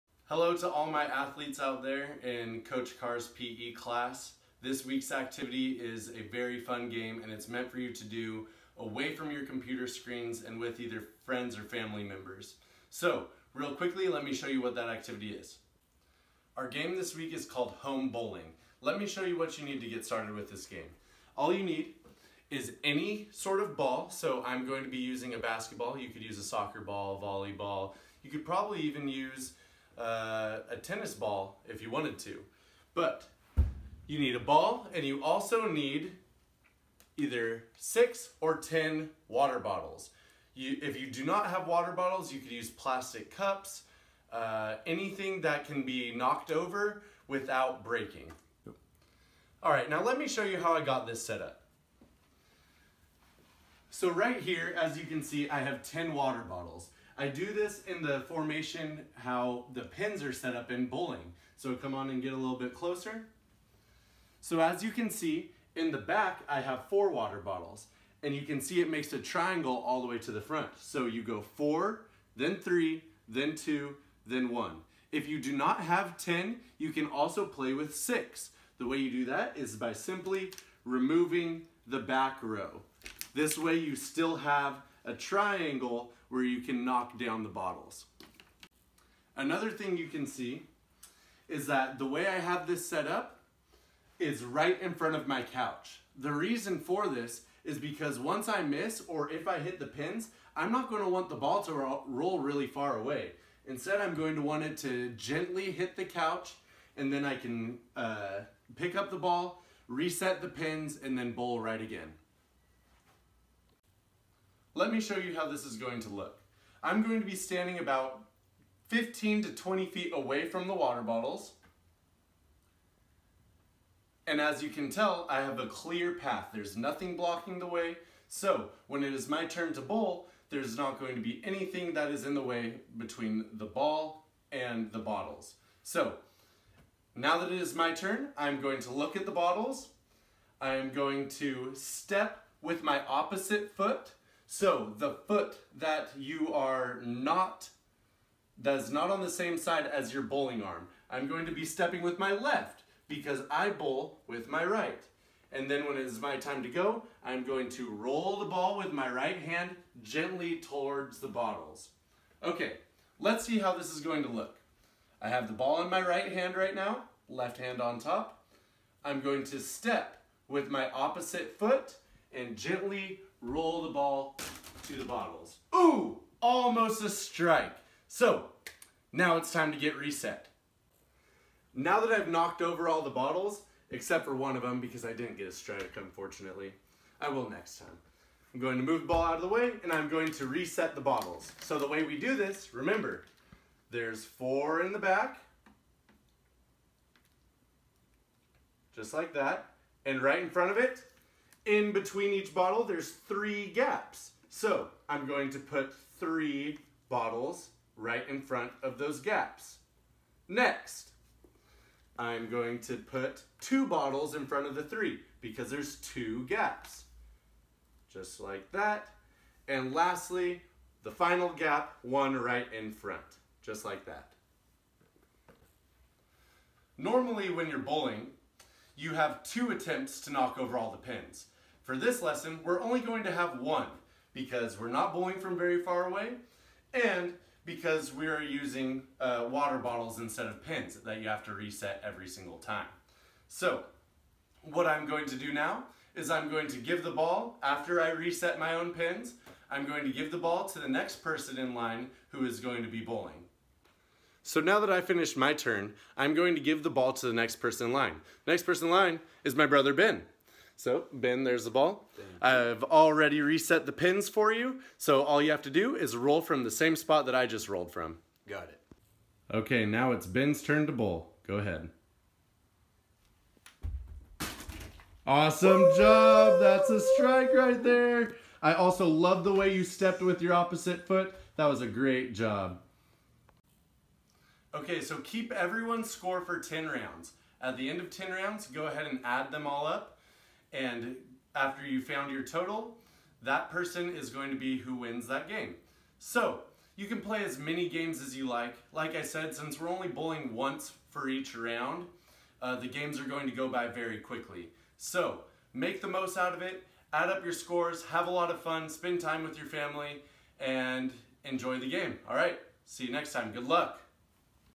Home Bowling by Coach Carr
Stuck in the house and looking for a game to play with the whole family? Why not play Home Bowling? Play for 10 frames, write down your score after each frame, and add up everyone's score at the end to see who wins! Good luck!